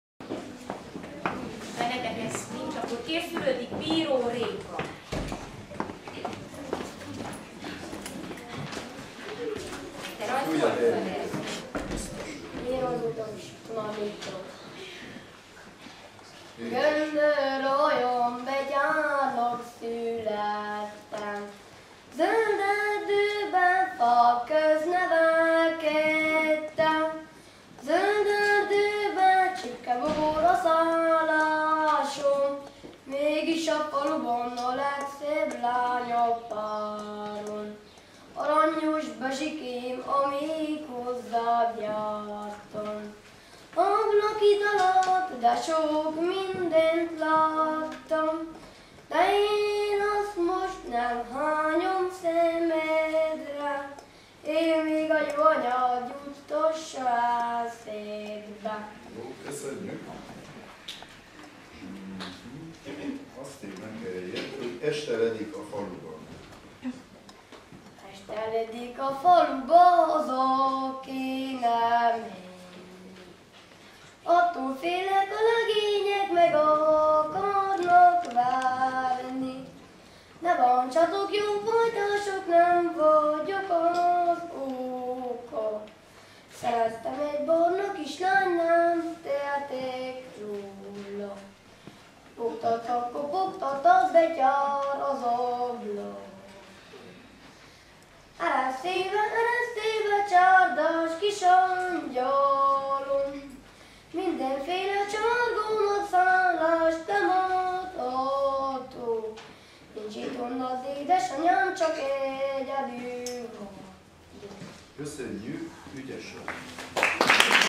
Bartha Ervin énekel
2009 - Bartha Ervin énekel a sepsiszentgyörgyi népdalversenyen.